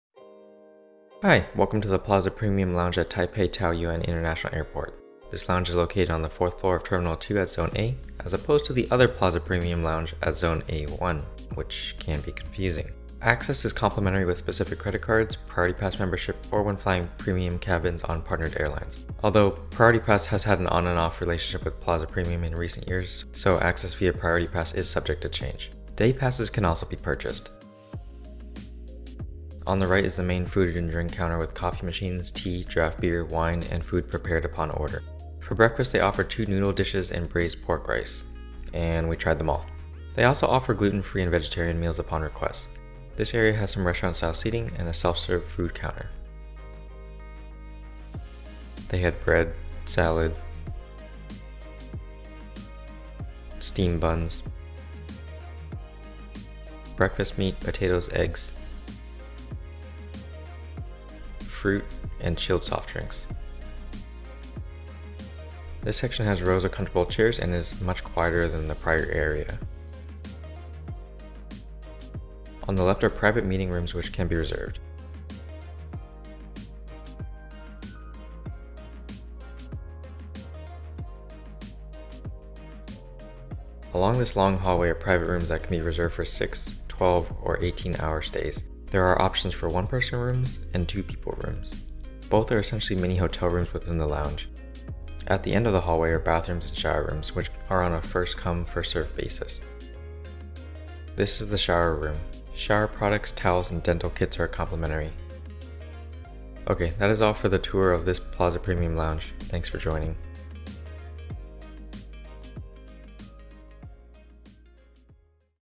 Plaza Premium Lounge at Taipei Taoyuan International Airport (TPE)
A tour of the Plaza Premium Lounge at TPE Terminal 2 Zone A

Location: Terminal 2, 4th Floor, Zone A
Hours: 5am - 11pm